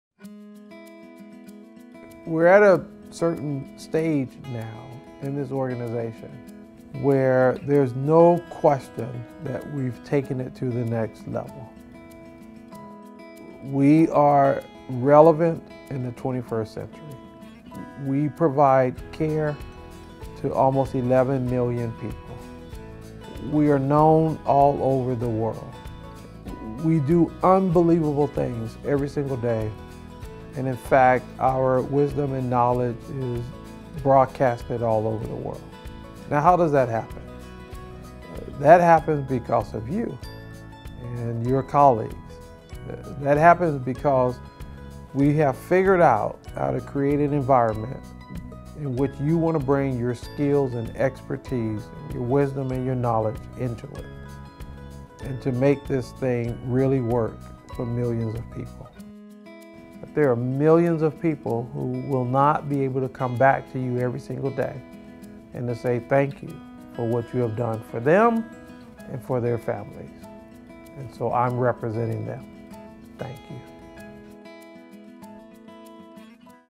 Heartfelt Thanks - Today and Always | Kaiser Permanente
This Thanksgiving, Kaiser Permanente Chairman and CEO Bernard J. Tyson wants to say thank you to the dedicated employees and physicians of Kaiser Permanente and to the almost 11 million people the organization serves. We are grateful for being recognized worldwide for our innovative approach to health care and for being able to help our members thrive. And that's because of the commitment and passion our employees bring to their work— today and every day.